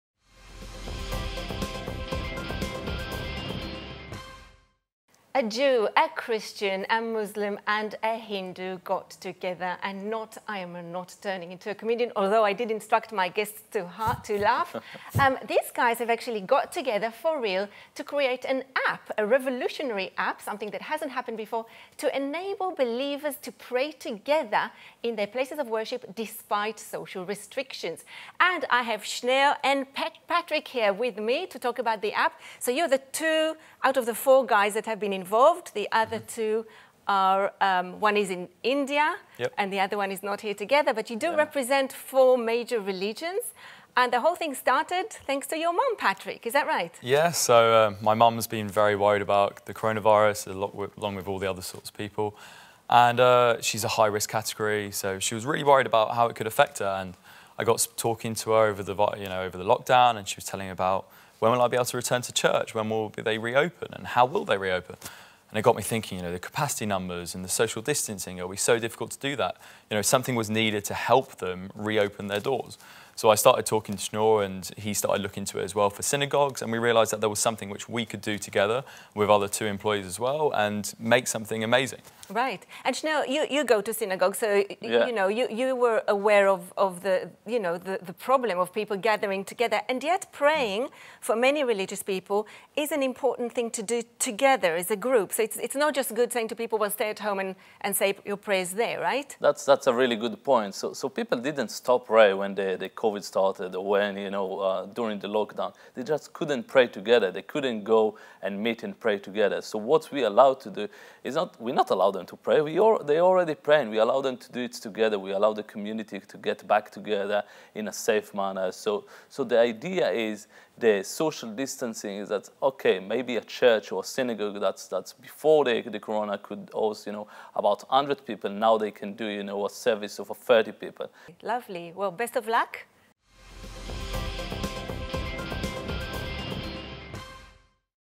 Introducing a New Prayer App for these times!
Latest TV Presenter, Yael Breuer, speaks to the people behind a new Prayer App. Shneor Crombie and Patrick Dobinson tell us all about it from the studio.